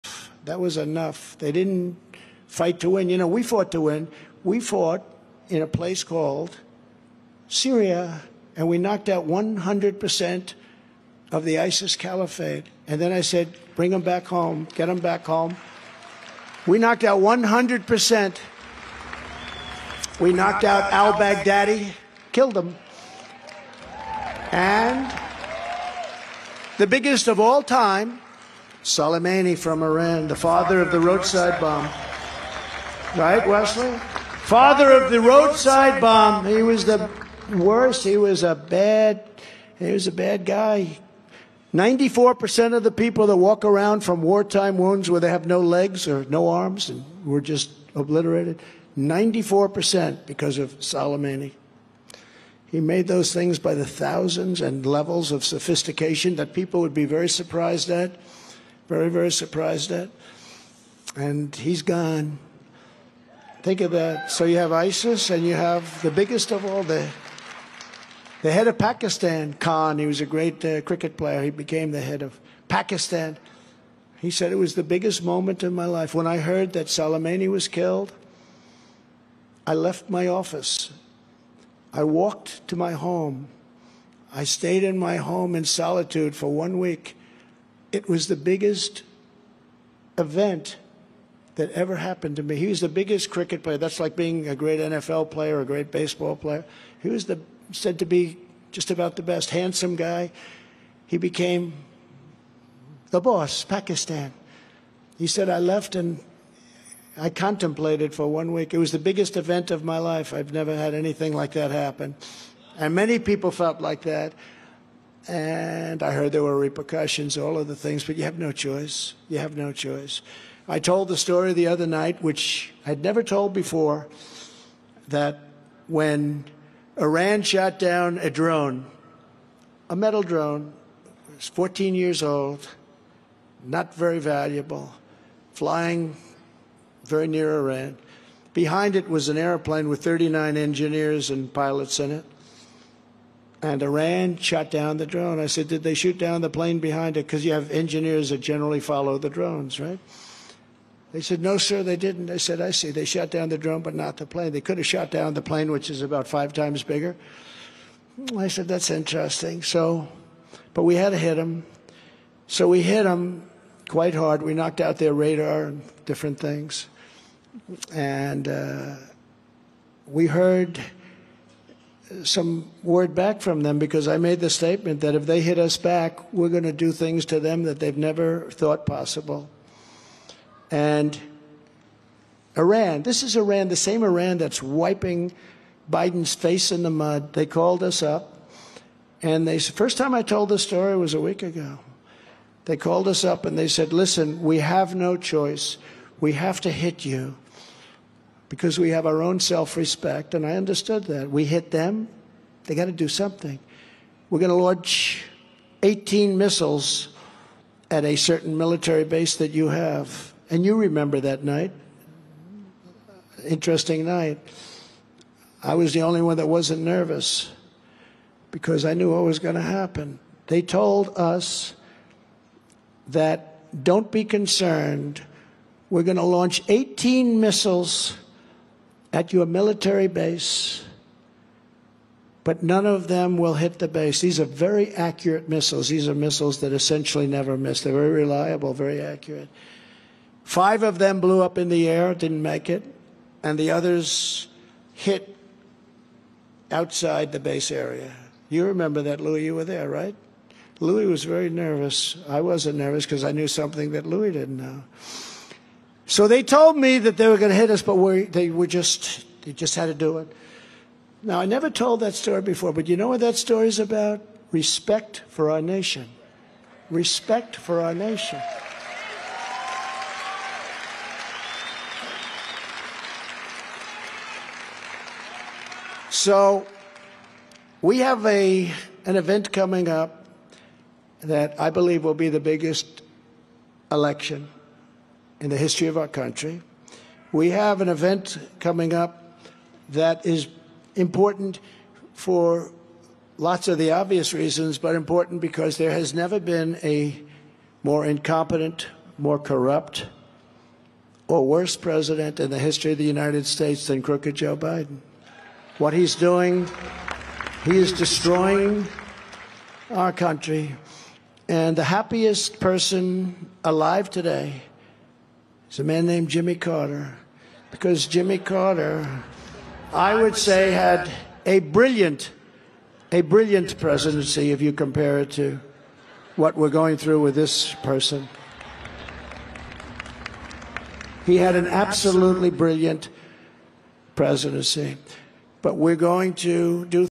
'The Same Iran That's Wiping Biden's Face In The Mud...': Trump Goes Off On Biden's Foreign Policy
At his rally last night in Houston, Texas, former President Trump slammed President Biden's foreign policy record.